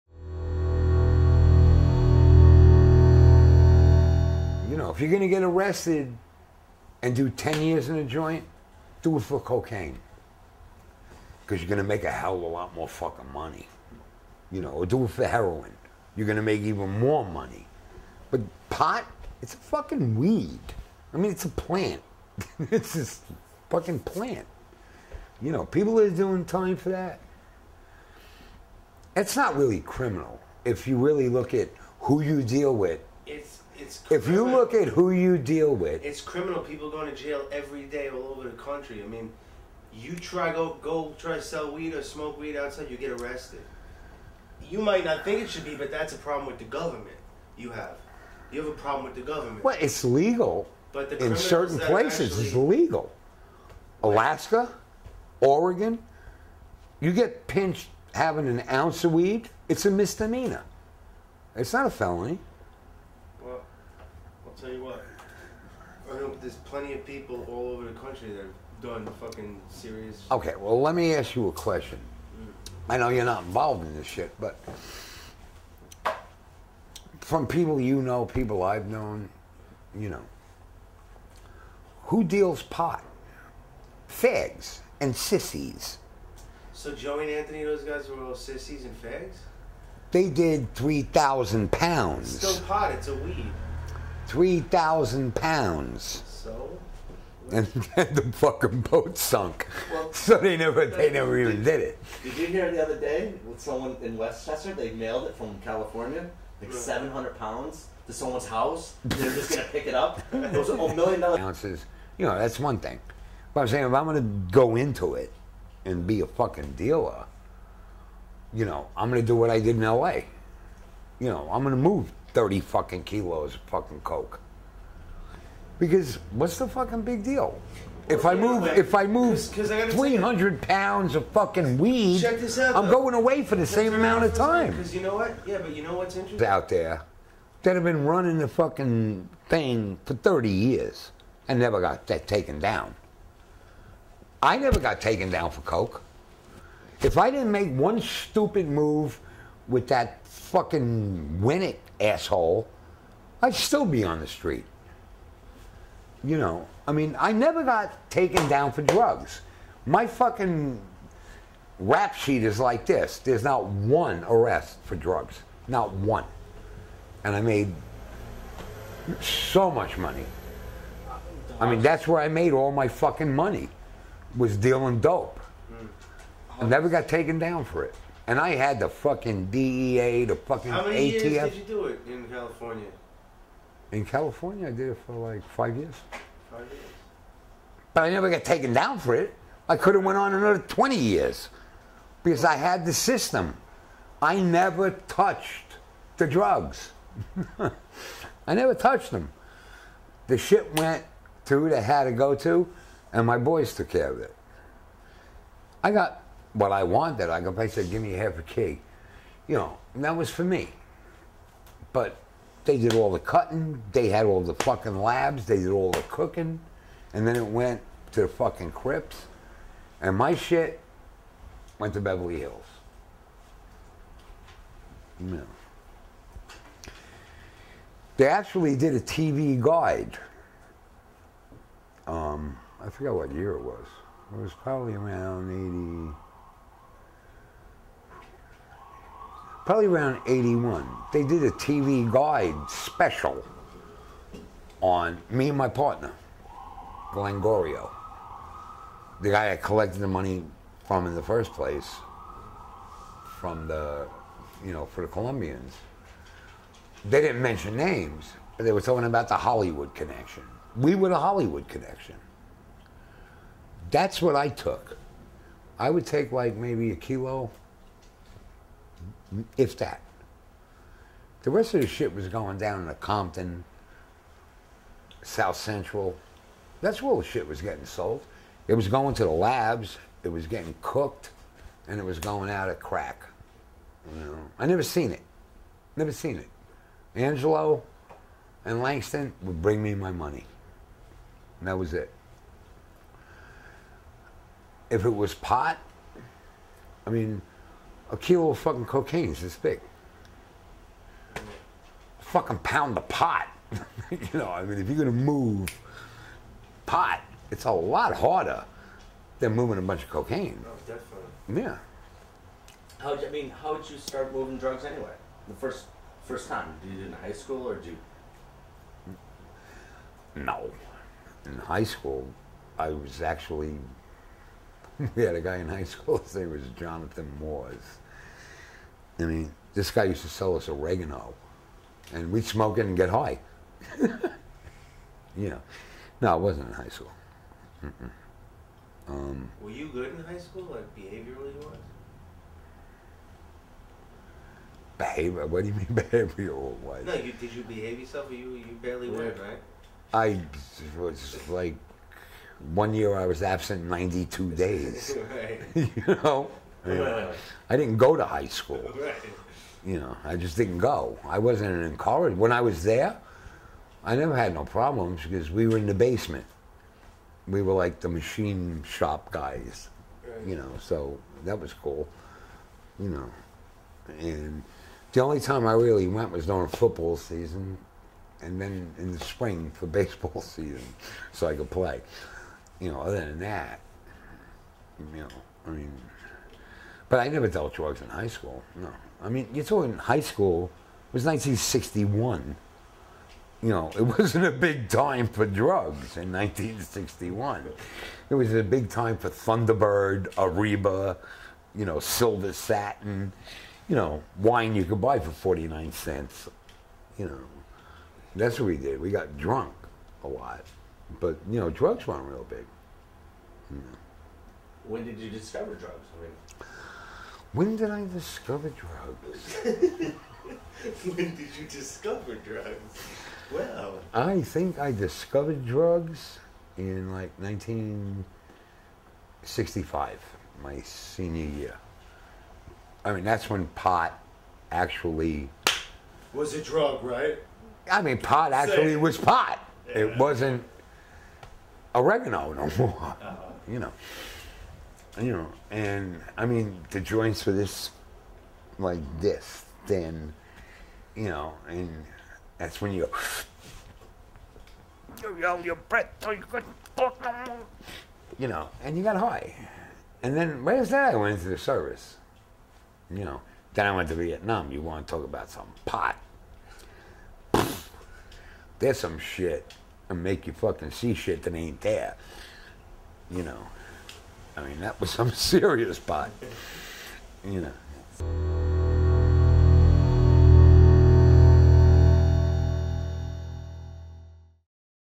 DOMINICK MONTIGLIO: Dealing Drugs
Dominick Montiglio discusses Dealing Cocaine, and the Crips